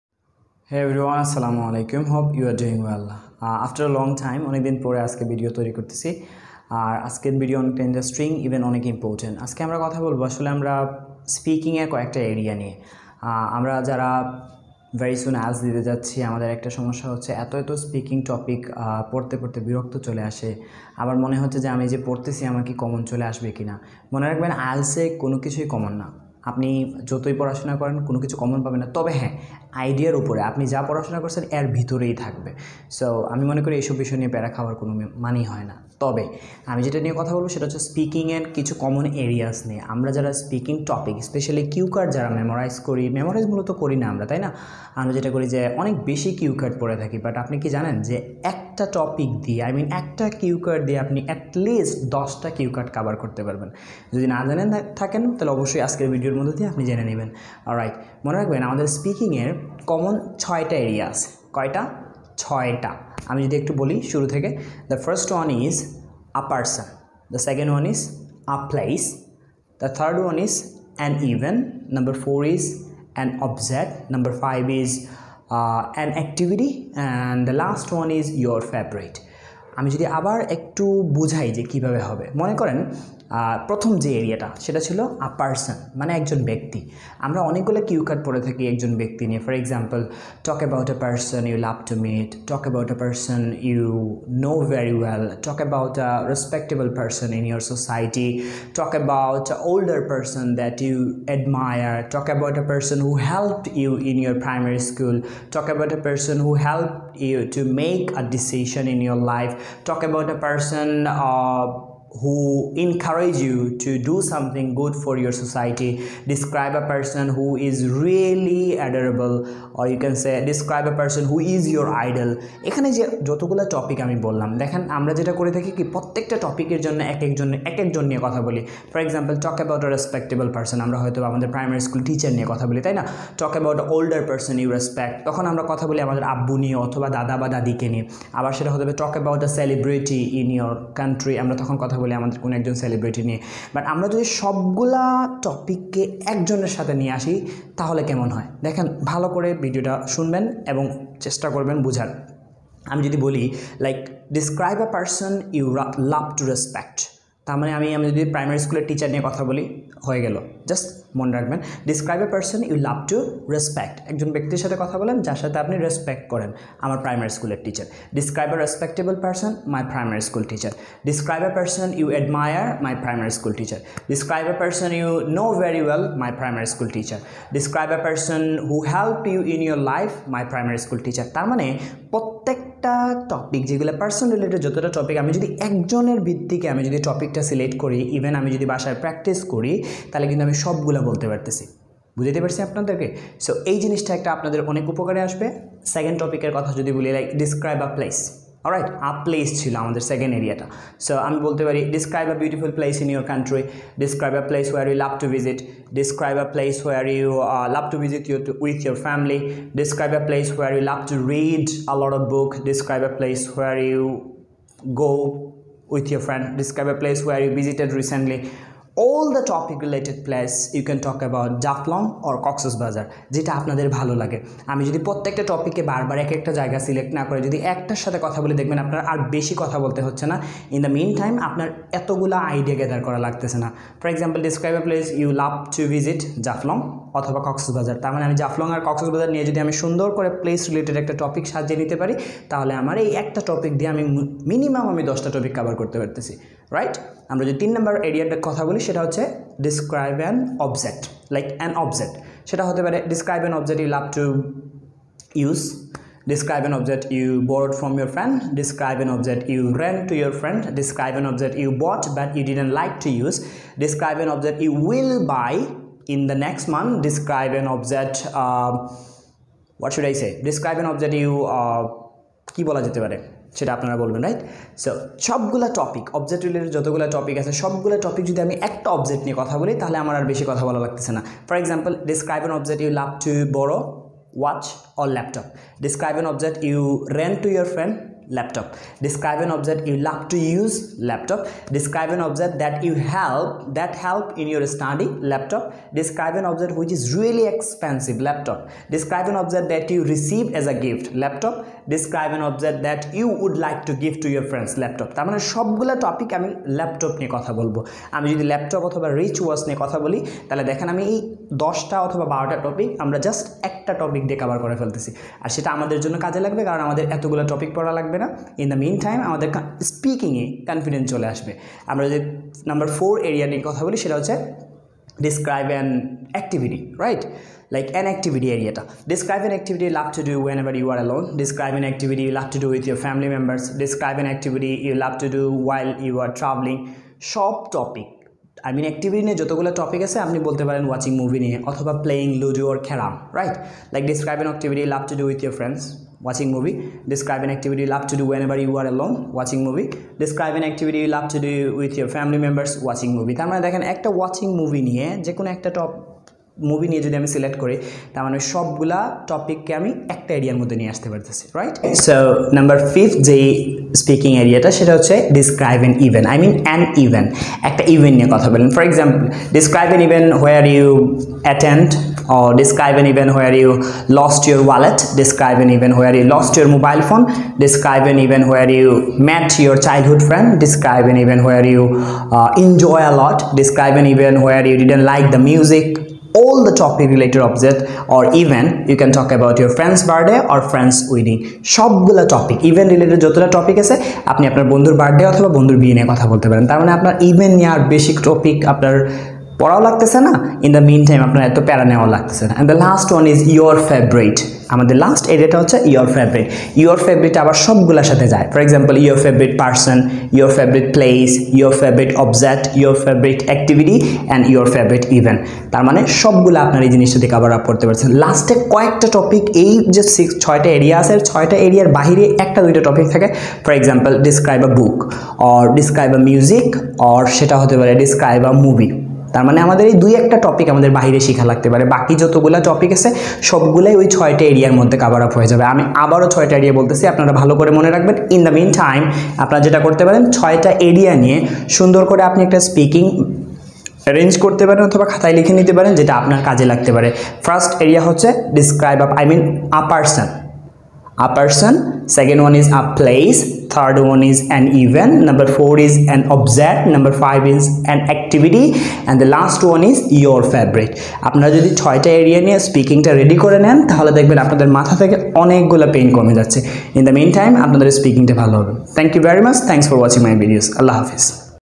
IELTS speaking | 6 areas of speaking part 2 | Mukul's World
Hi there, this video will help you get an idea of speaking part 2. You can make note of these areas and try to practice them every day.